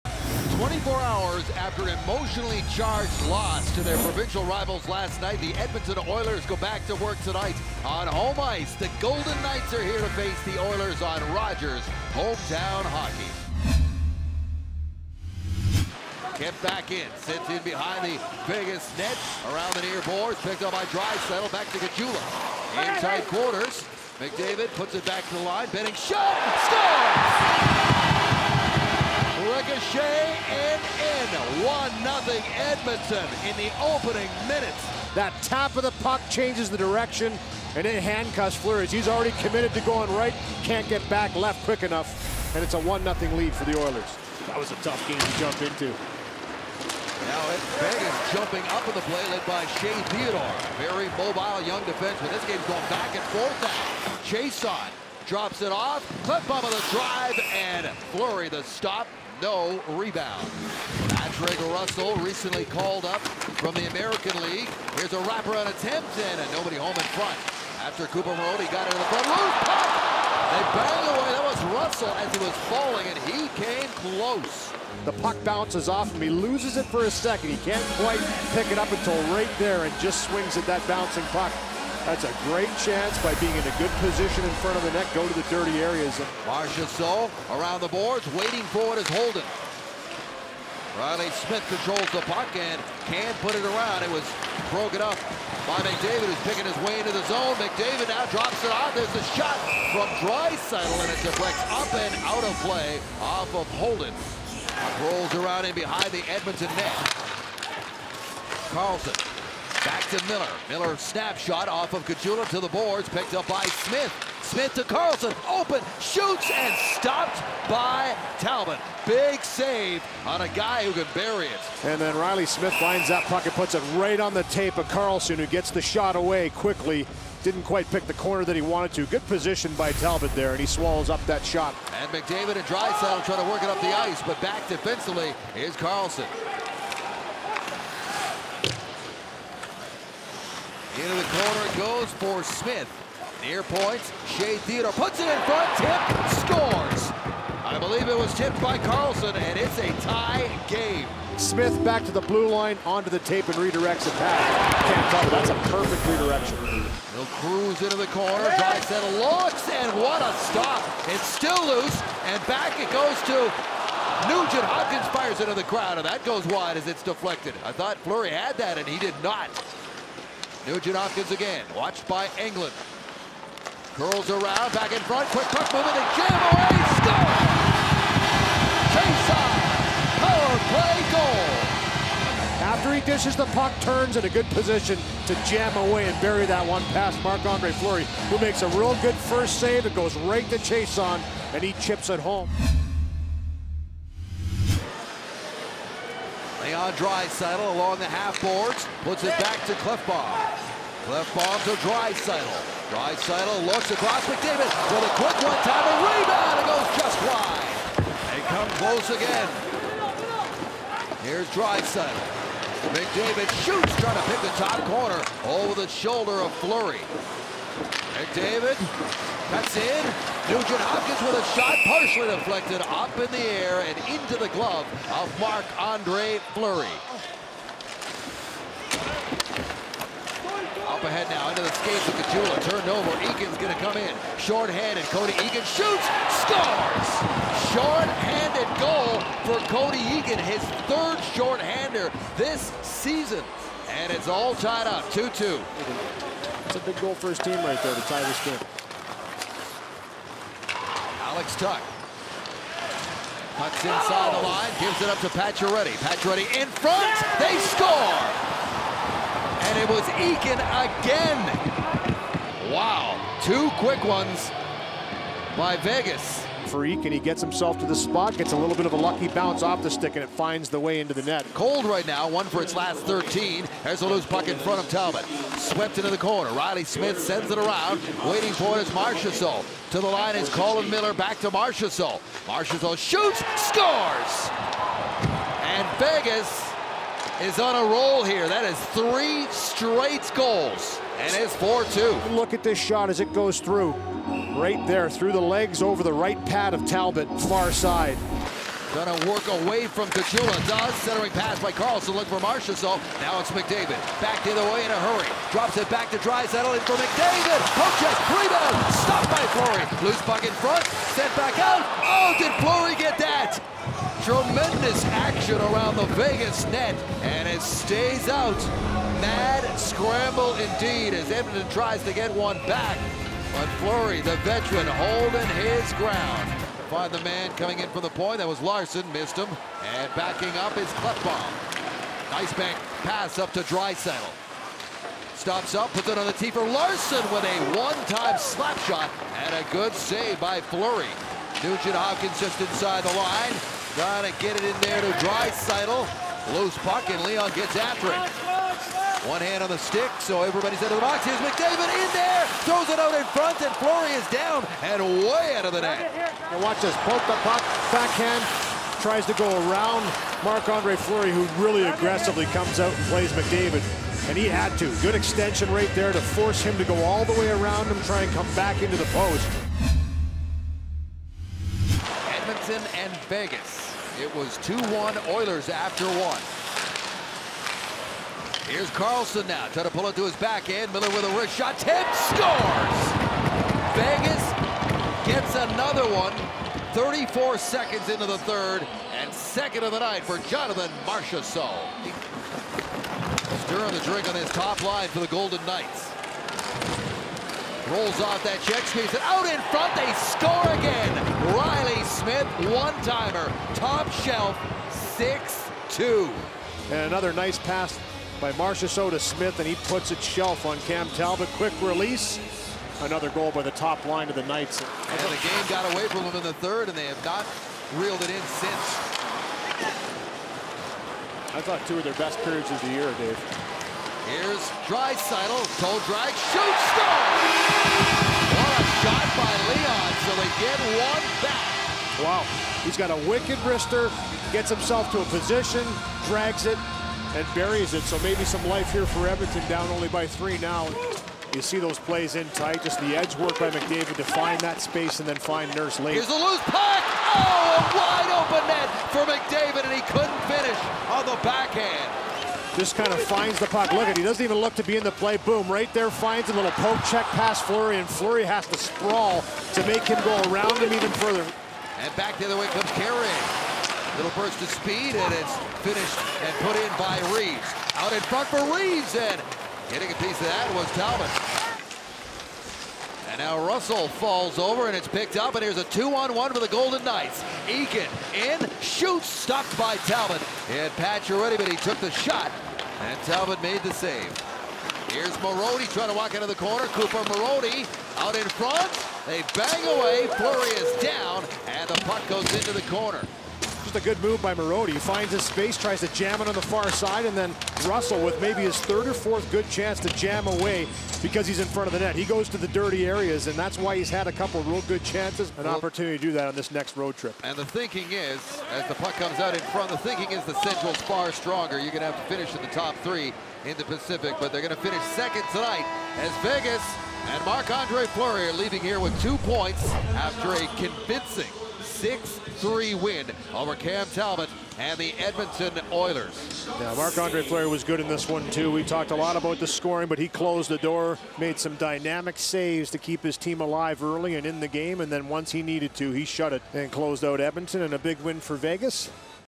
11/18/18 Condensed Game: Golden Knights @ Oilers
Extended highlights of the Vegas Golden Knights at the Edmonton Oilers

Feeling social?